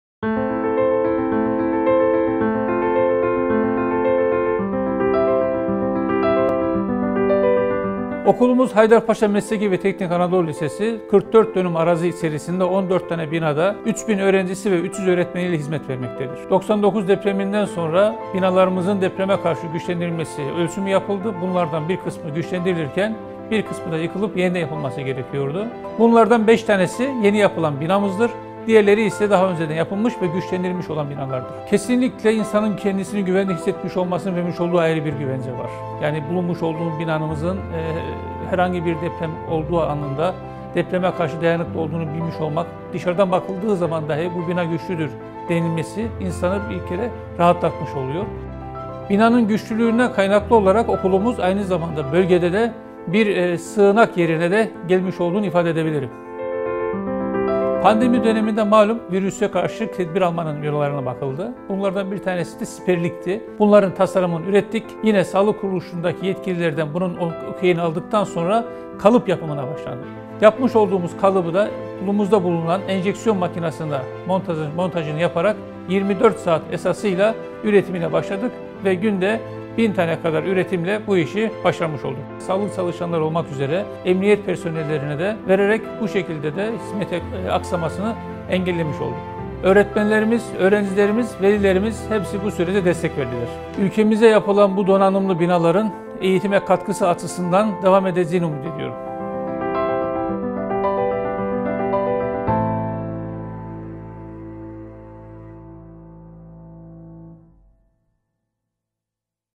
The Schools Reconstructed by IPCU: Haydarpaşa Vocational and Technical Anatolian High School
Istanbul Governorate has taken an active role in the production of necessary equipment during the COVID-19 pandemic in vocational schools, which were renewed or rebuilt within the scope of the Istanbul Seismic Risk Reduction and Emergency Preparedness Project (ISMEP), implemented by the Istanbul Project Coordination Unit (IPCU). 200 Vocational High Schools support the production cycle with a staff of approximately 1000 people!